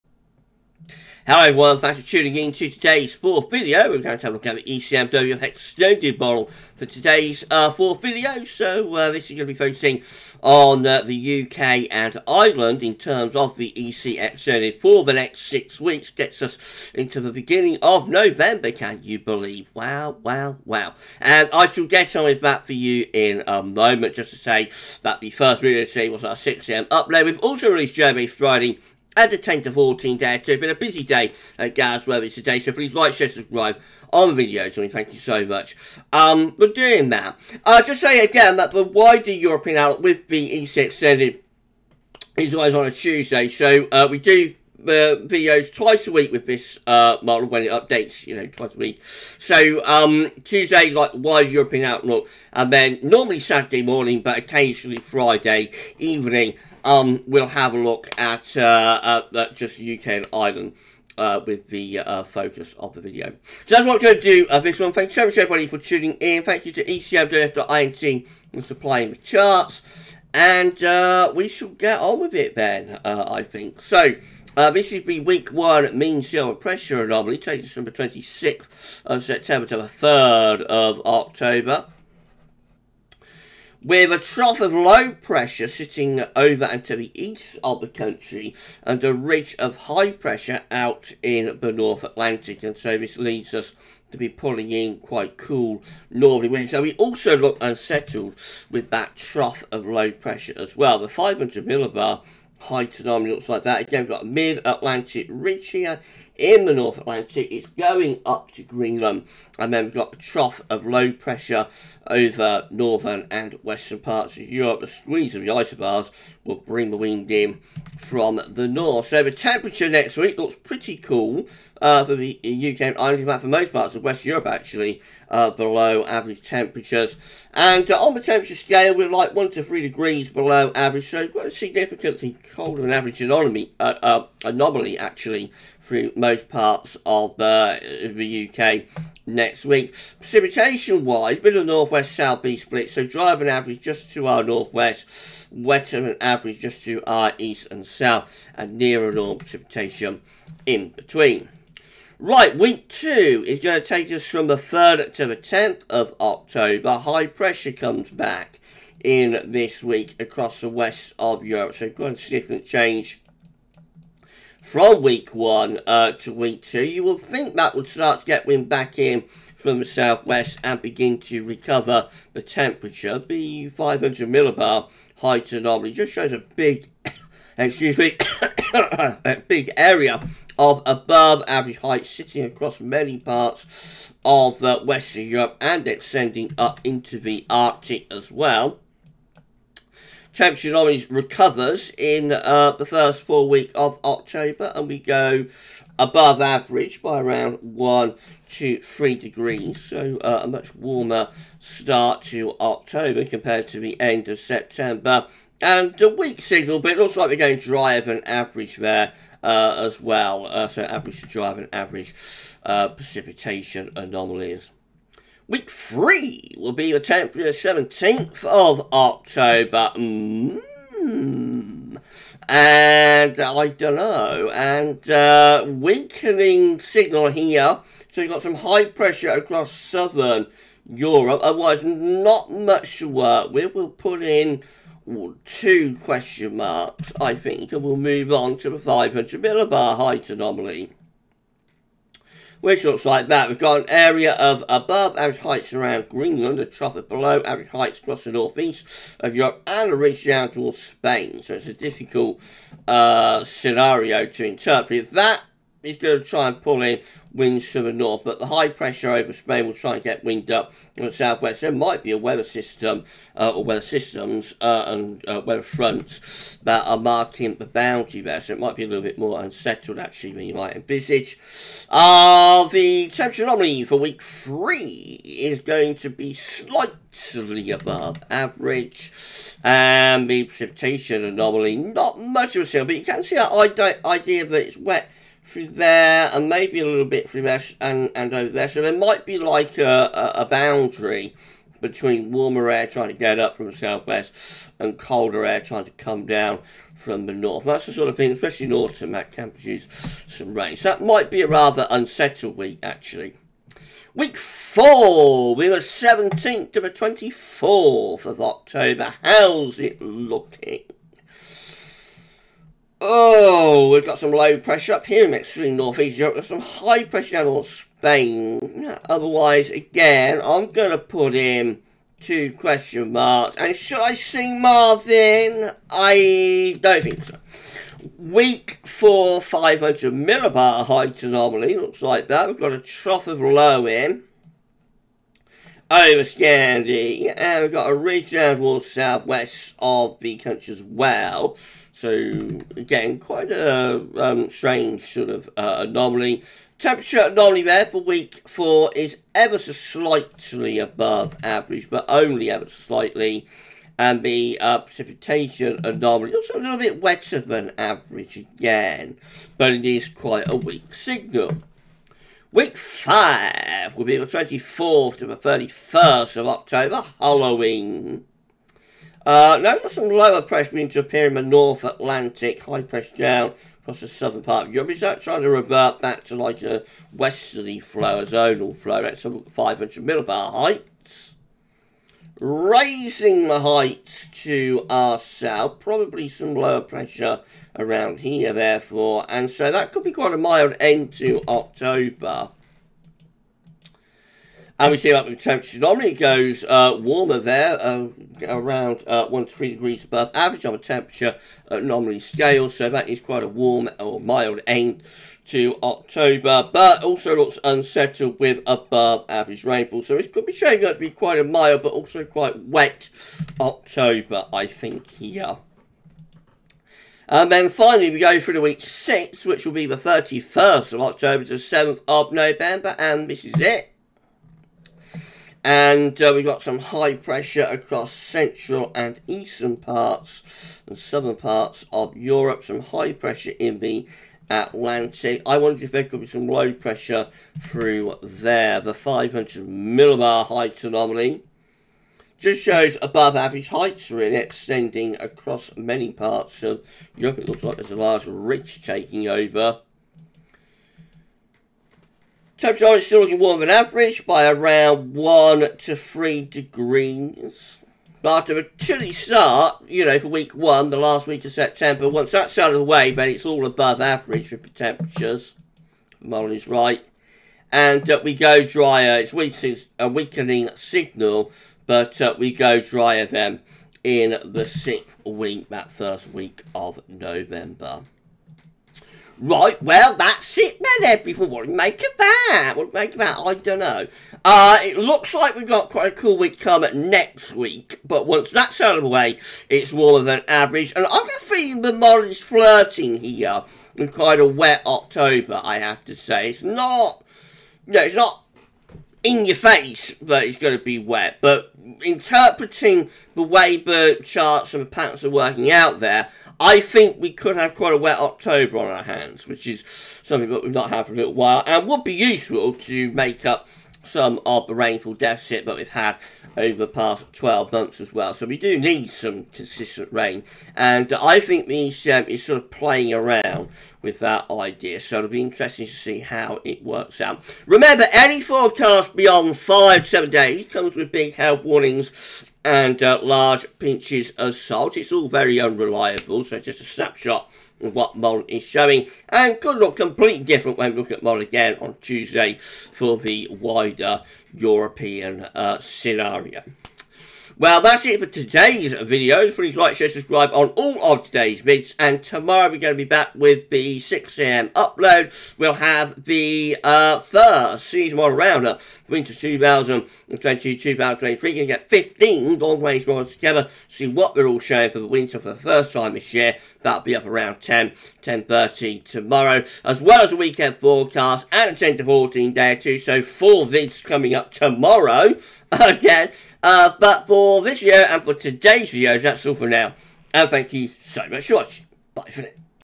ECMWF 42 Day Forecast: Wet October On The Way?
Welcome to this weeks extended ECMWF 42 day forecast for the UK and Ireland.

This video forecast from the extended ECMWF model takes us from 26th September to 7th November 2022.

💡  Video Credits: 💡

 💡 GWV Links: 💡